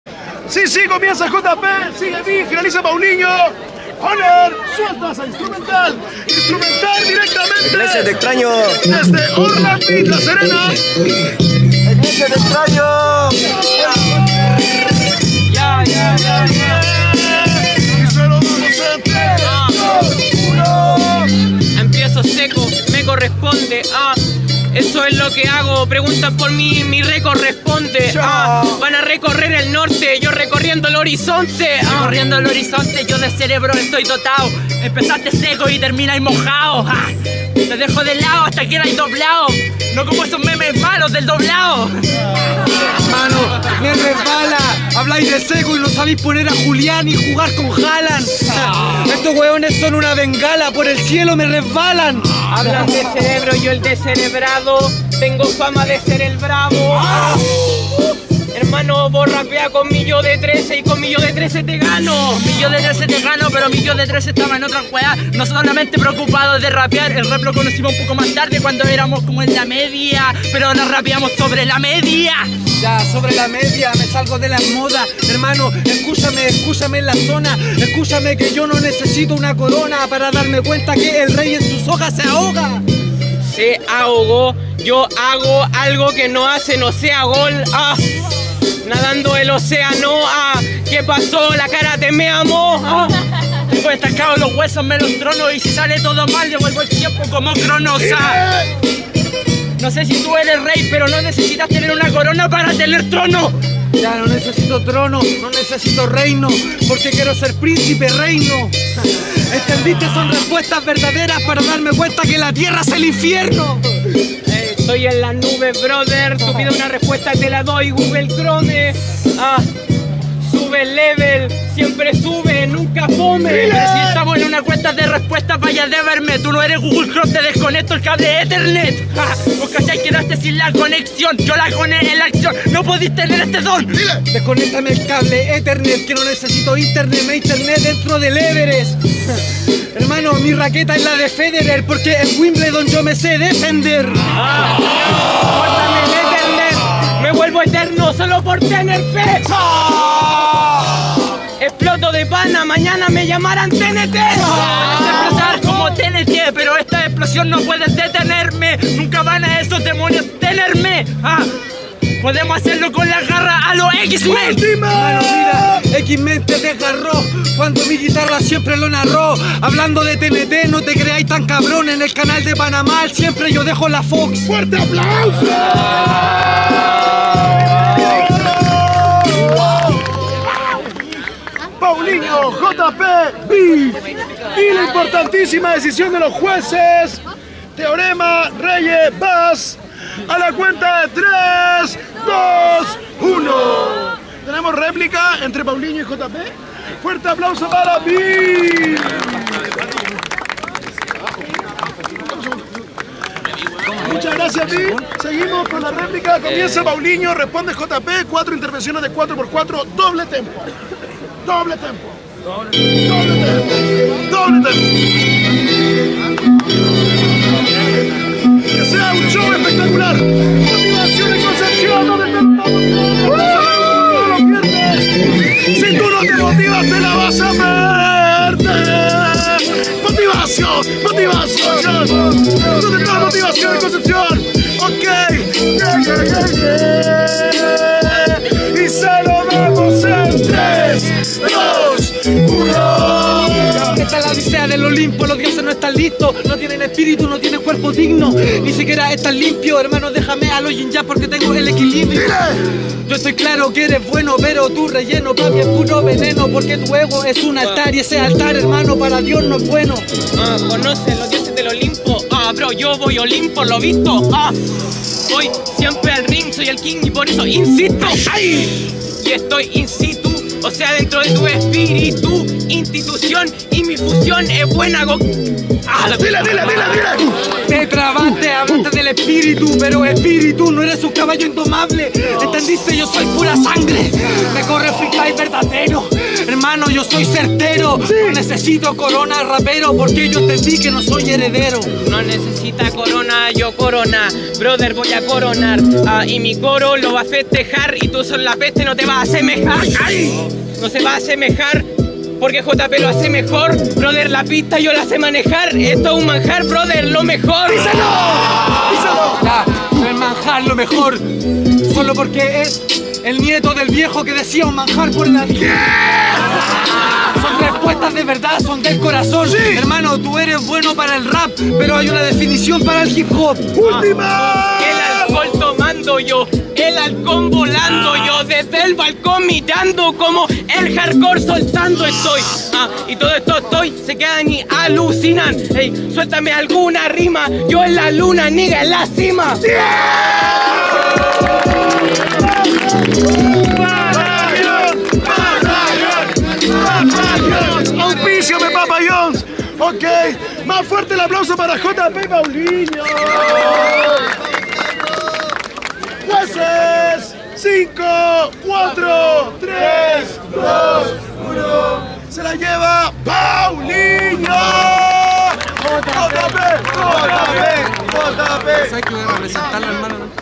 JP vs. BE vs. PAULINHO: Octavos - Liga Inmortal Apertura 2024 | Fecha 22
Liga Inmortal Apertura 2024 | Fecha 22 - 09 de Agosto 2024
📍Plaza Perú, Concepción 🇨🇱

Liga de freestyle todos los viernes en Plaza Perú Concepción, Chile 🇨🇱